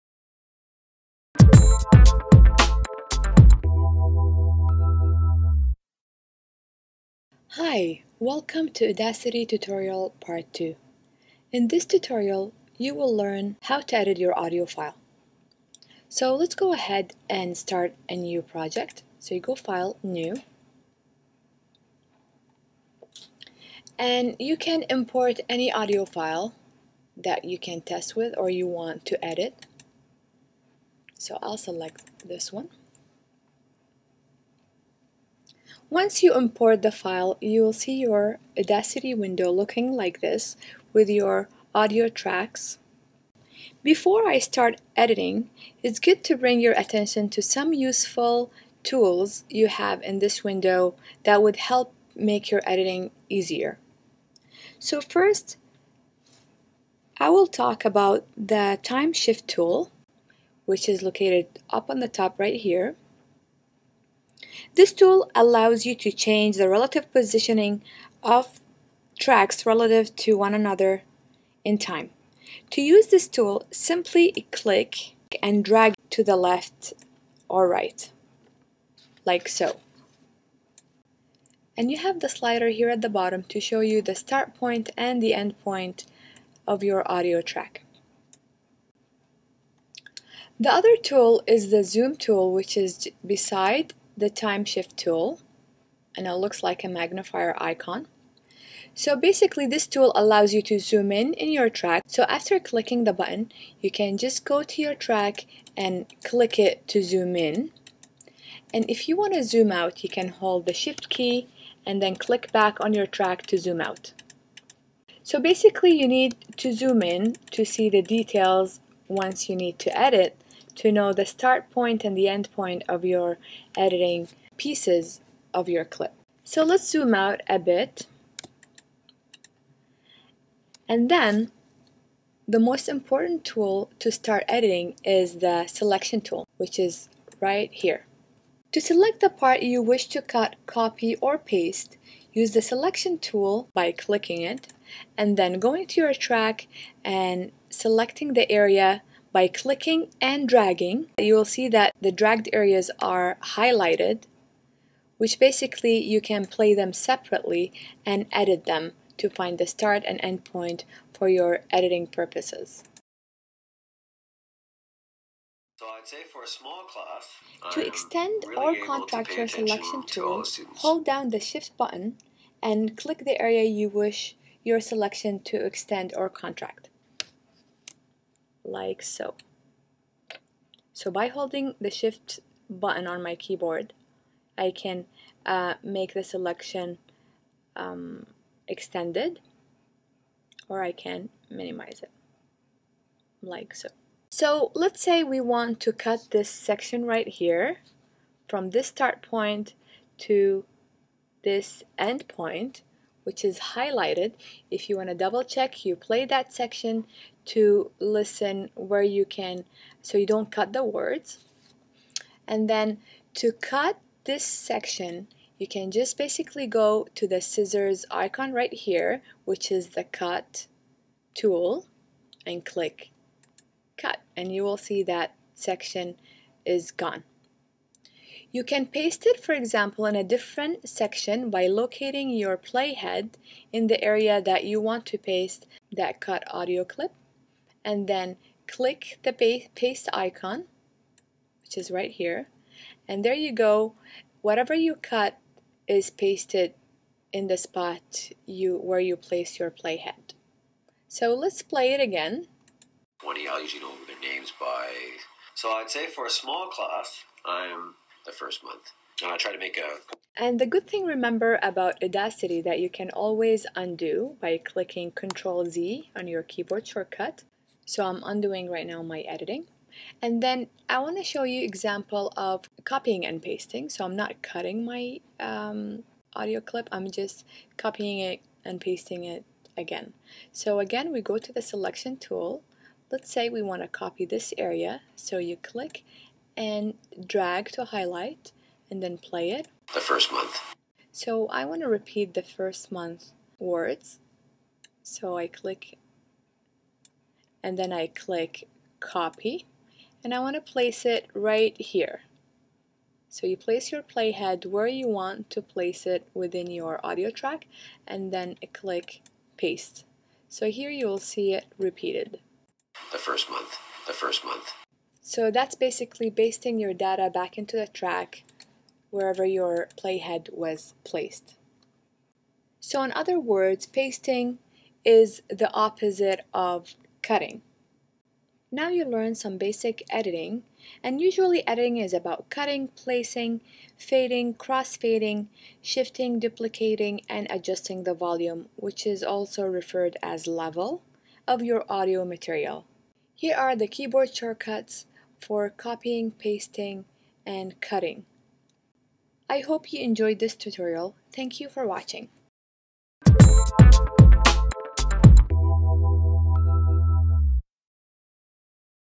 Audacity Tutorial 2: Audio Editing
The video contains a quick step by step demo on how to edit an audio using Audacity software.